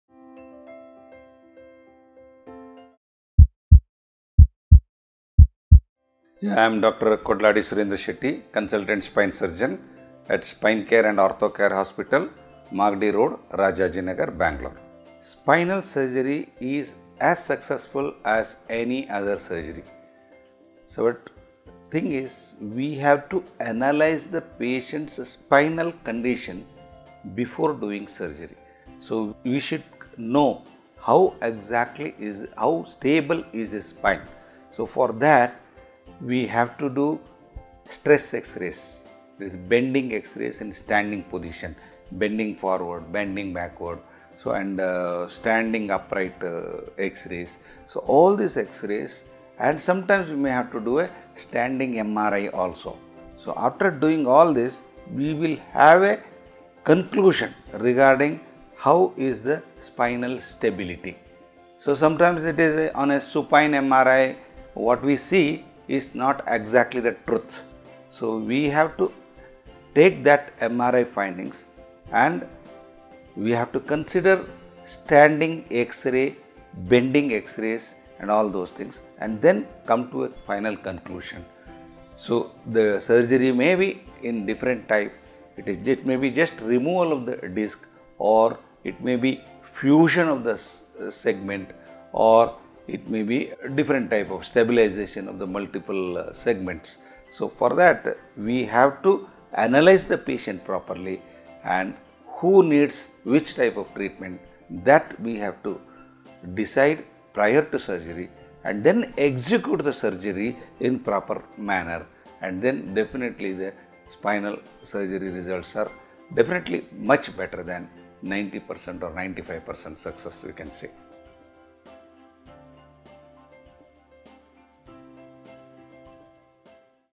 What is the success rate of spinal surgery? - Dr. Kodlady Surendra Shetty
Spinal surgery is as successful as any other surgery.   We have to analyse the patients  spinal condition before dong the surgery. We should   know how stable is the spine. For that we should do the stress x-ray, that is the bending x-ray in standing position. Bending forward, bending backwards, standing upright xrays. All this and sometimes and sometimes w e have to do a  standing MRI  also.  After doing all this we will get a conclusion regarding how is the spinal stability. On a supine MRI what we see is not the truth.  We have  to take t he MRI findings. We have to do standing x-rays, bending x-rays and then come  to a  final conclusion. The surgery may be in different  types, it may be removal of the disc, or  fusion of the segments  or deferent types of stabilisation of multiple segments, for that we have to  analyse the  patient properly and  who needs what type of treatment, that  we have  to decide prior to surgery and  execute the  surgery in proper manner and finally  the Spinal surgery  results are much better than  90%  or 95% success.